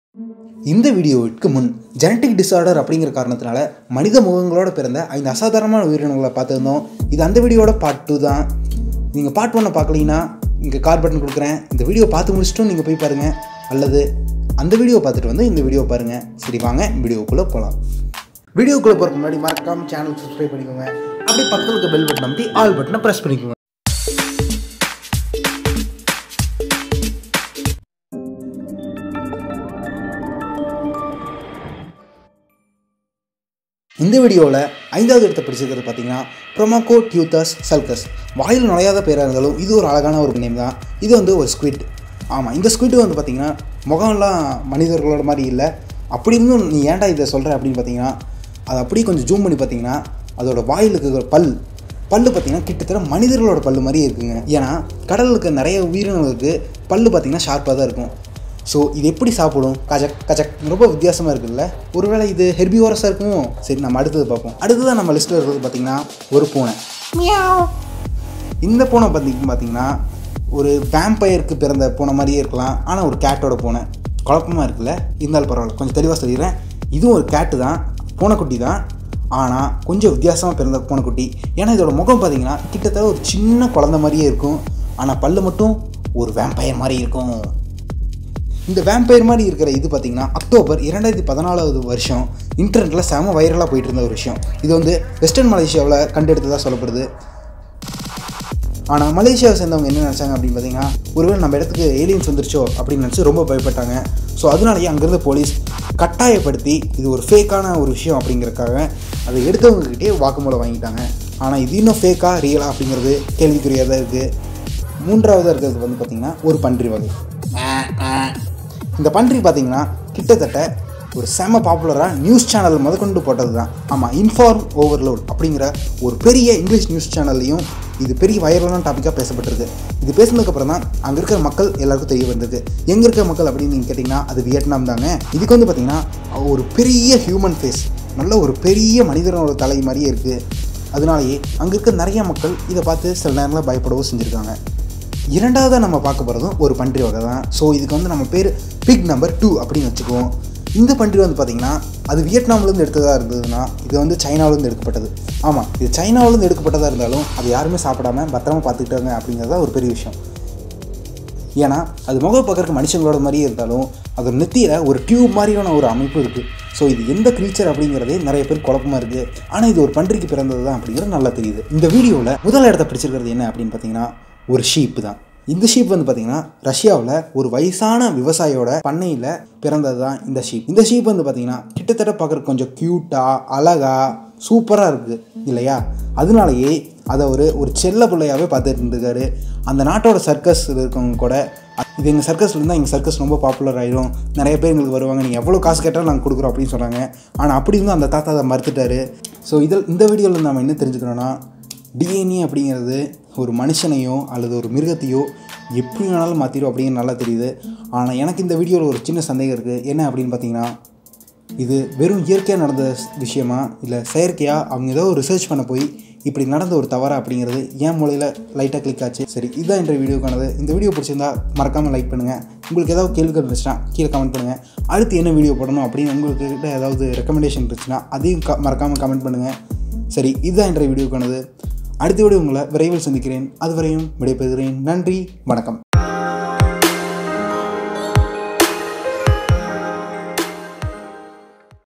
அடேங்கப்பா! மனித முகமா? | Part 2
TOP 5 Animals born with human Head | அடேங்கபா! மனித முகமா? | Part 2 |SCIENTIFIC TARGET

They are Not a Diiferent Creatures But,It's Completely Natural,Because it's Mainly Happened Due to Genetic Disorder.